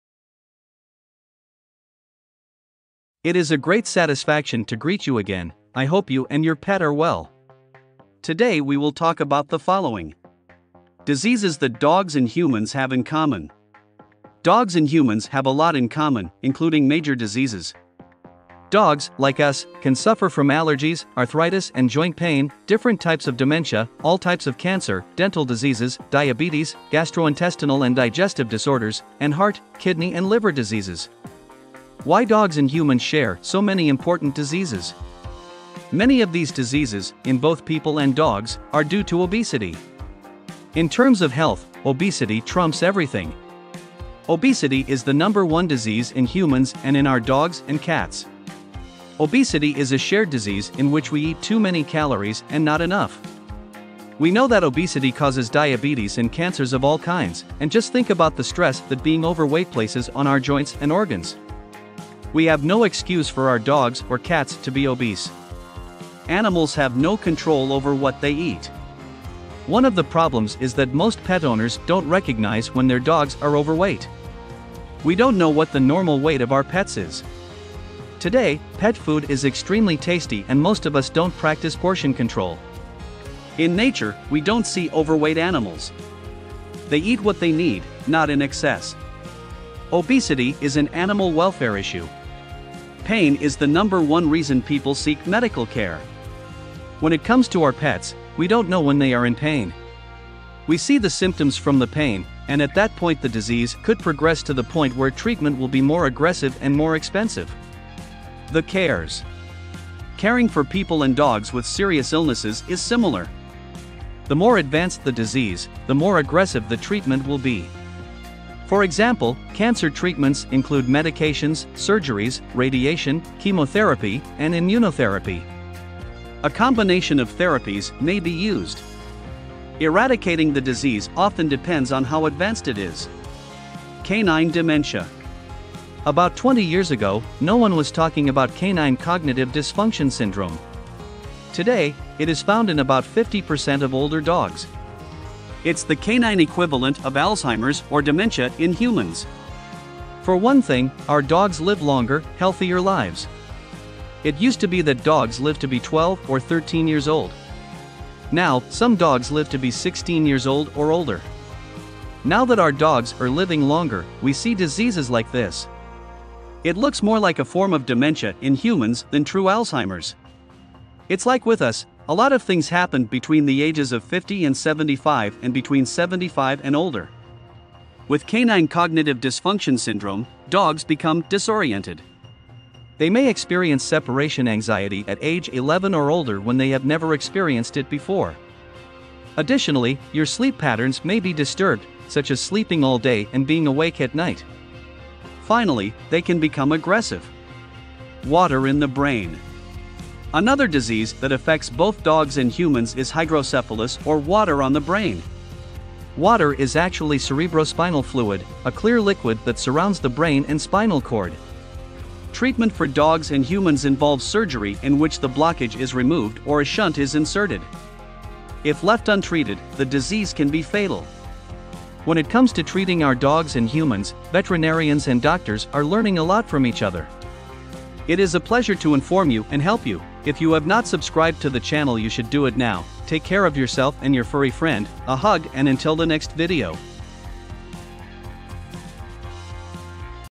DISEASES that DOGS and HUMANS have in COMMON🐶🧑
📌Dogs and humans have a lot in common, including major diseases. Dogs, like us, can suffer from allergies, arthritis and joint pain, different types of dementia, all types of cancer, dental diseases, diabetes, gastrointestinal and digestive disorders, and heart, kidney and liver diseases.

Why dogs and humans share so many important diseases... WATCH THE VIDEO.